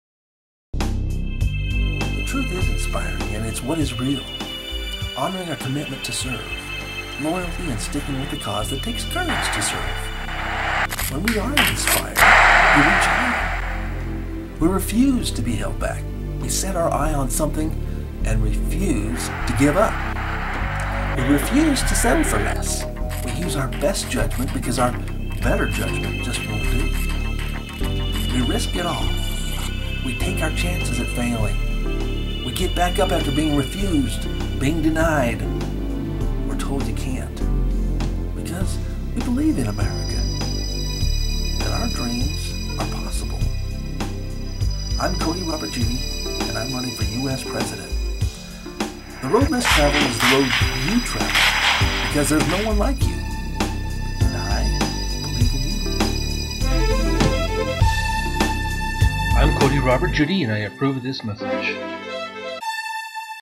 INSPIRING- CRJ 2016 Presidential Commercial
INSPIRING - I believe in You- One of the best things I feel deep in my heart is that opportunity exist in America that is protected under our United States Constitution. Those who have capitalized on that opportunity need to appreciate what they have been given.